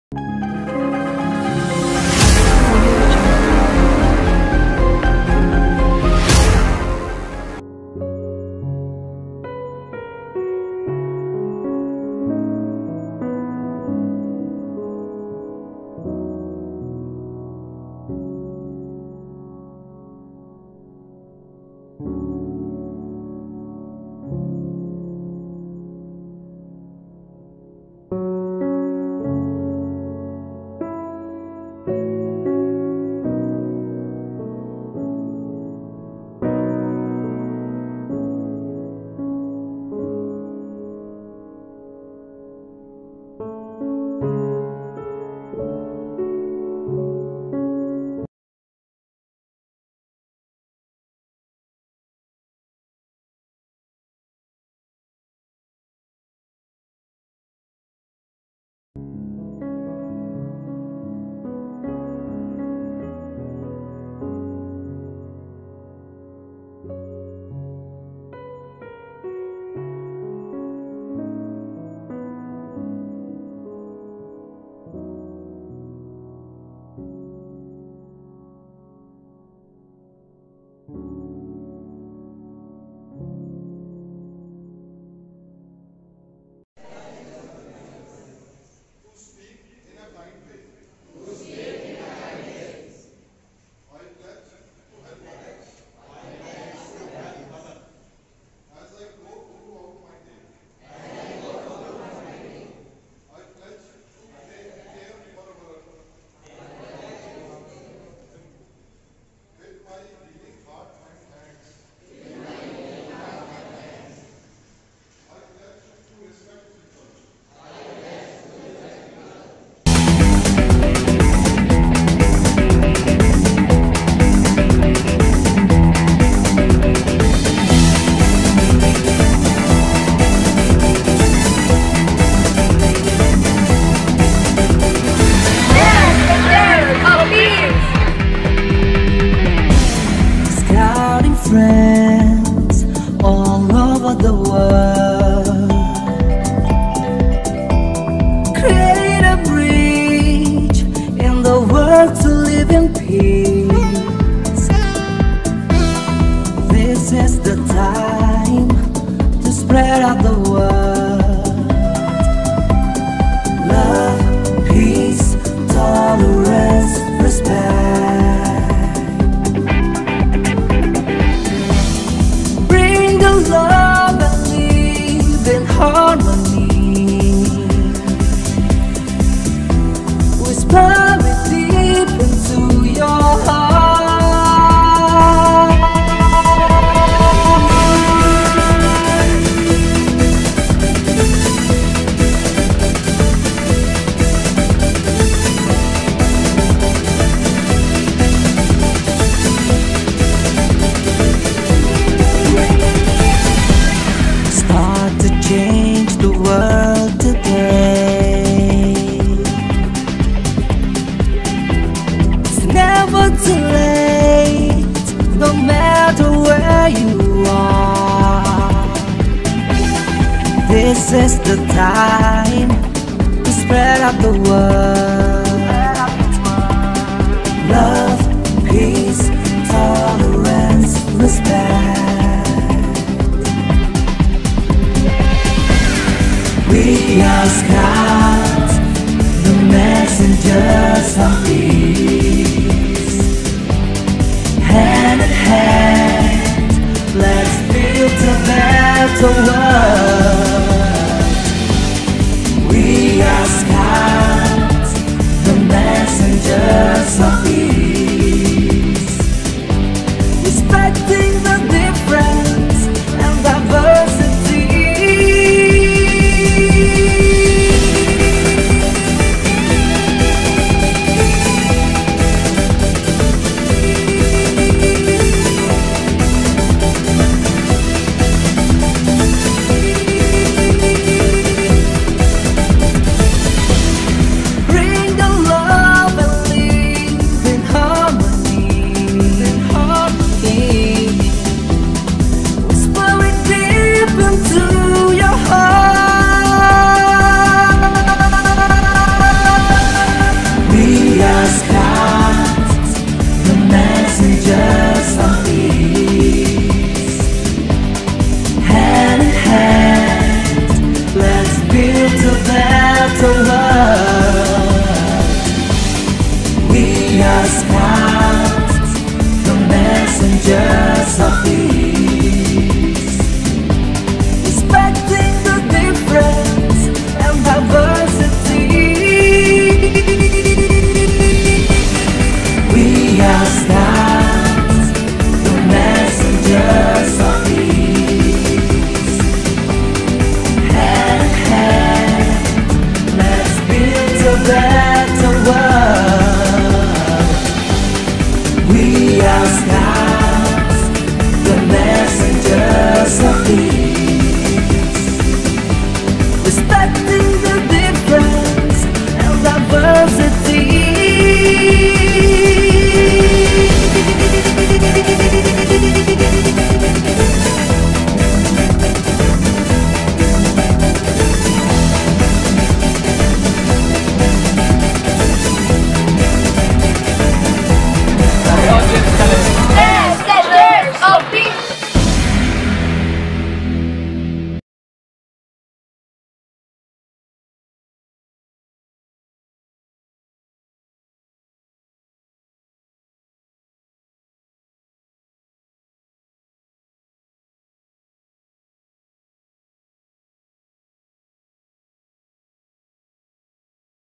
INTERNATIONAL PEACE DAY CELEBRATION 2021
The peace day celebration during a camp on district headquarters Sri Ganganagar , Rajasthan.

1. Scout Guide Academy  (Facebook)  :-

2. WhatsApp Channel :-

3. Yatin Verma   (Instagram)  :-